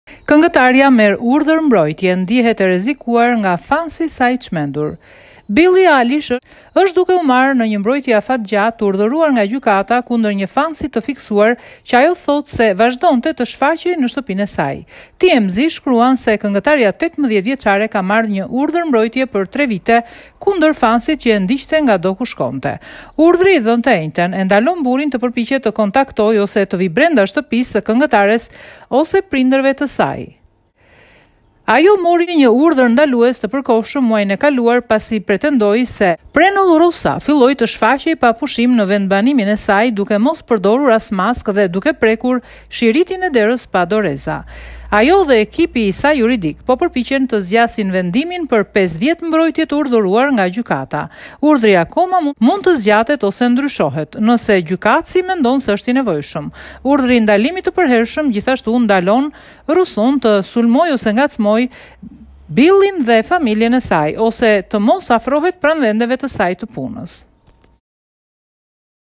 Këngëtarja merr urdhër mbrojtje për t'i shpëtuar fansit të fiksuar pas saj
Billie Eilish, iu desh të merrte një urdhër mbrojtje për t'i shpëtuar një fansi të fiksuar pas saj!  Sipas TMZ-së, këngëtarja "ka një urdhër të gjatë mbrojtjeje" kundër këtij njeriu që i shfaqet krejt papritur, i paftuar edhe në shtëpi.